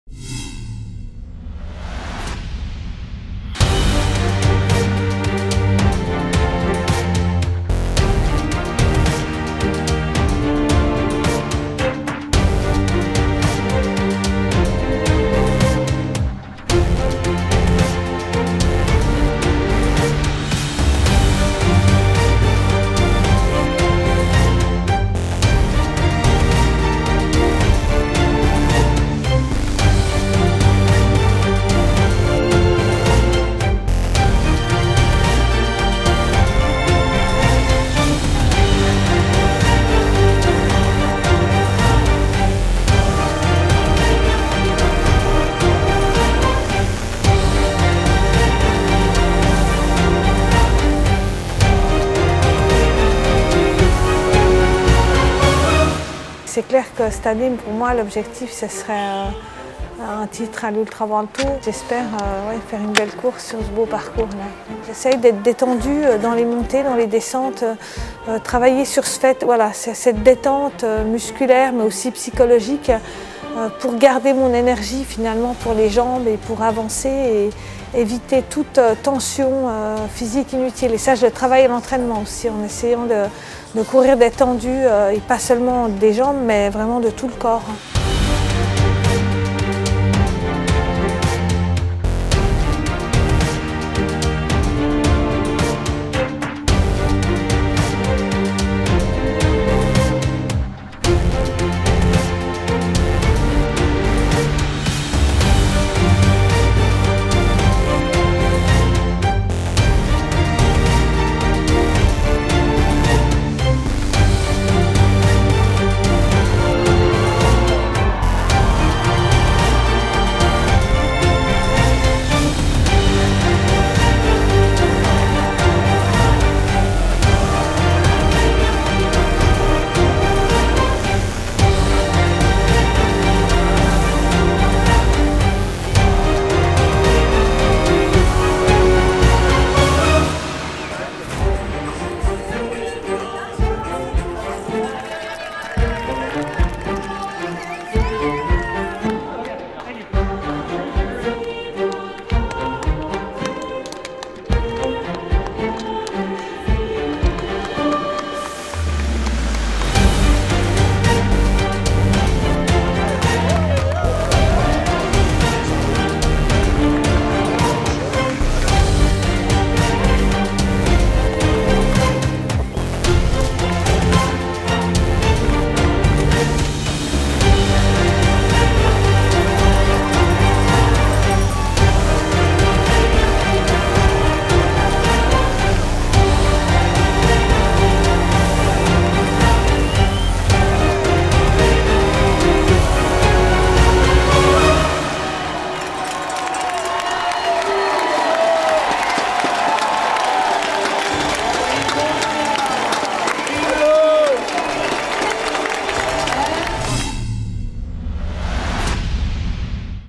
Caroline Chaverot - Ultra-Trail World Tour highlights 2016
Congratulations to Caroline, winner of the Ultra-Trail World Tour after victories at Transgrancanaria HG, Madeira Island Ultra-Trail and UTMB!